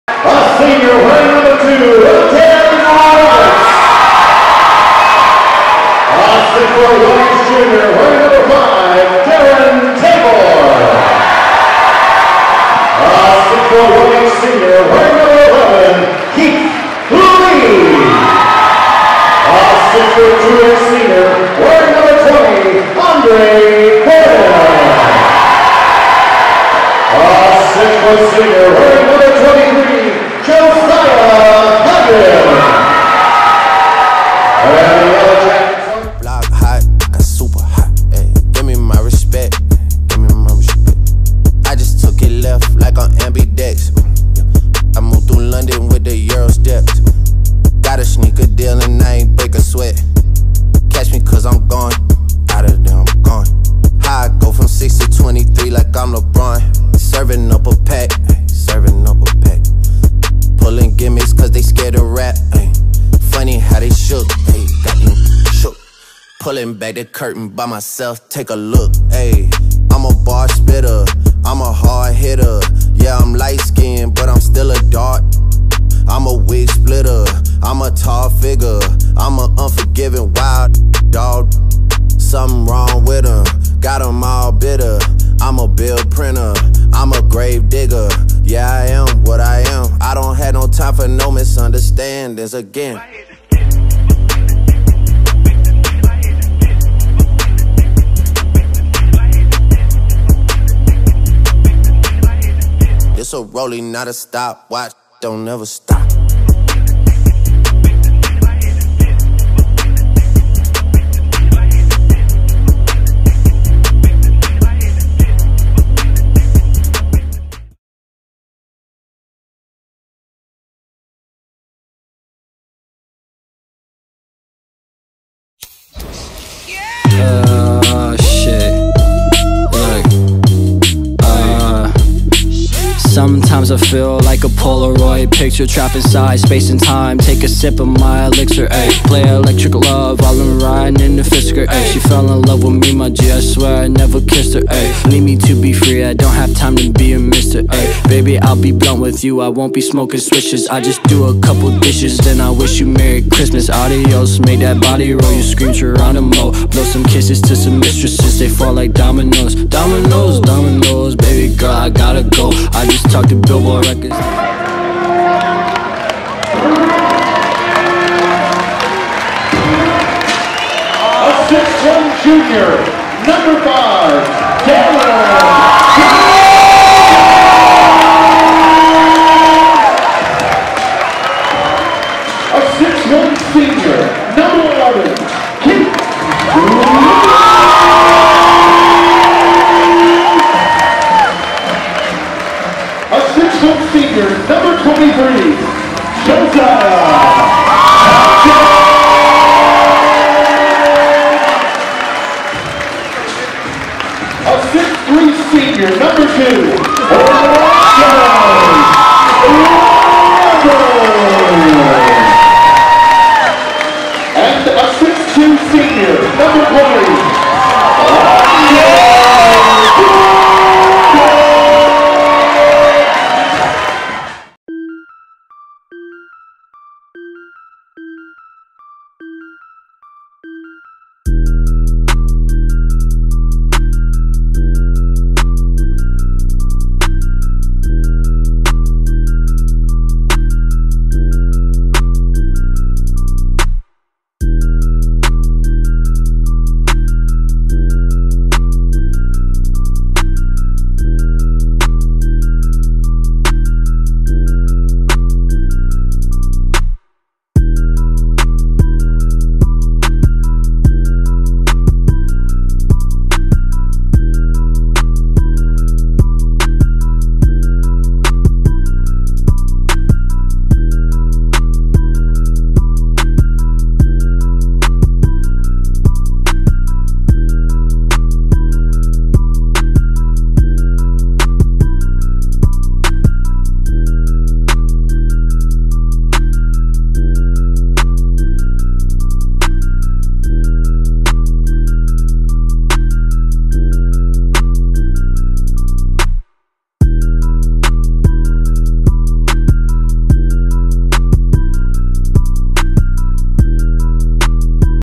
Sidney Varsity Basketball Highlights ft. Andre Gordon (Reupload)
Sidney @ rival Piqua, Butler @ Sidney for GWOC North Title
Featuring: Virginia Tech University Commit, Andre Gordon, Urbana University Commit, Ratez Roberts and Tiffin University Commit, Josiah Hudgins.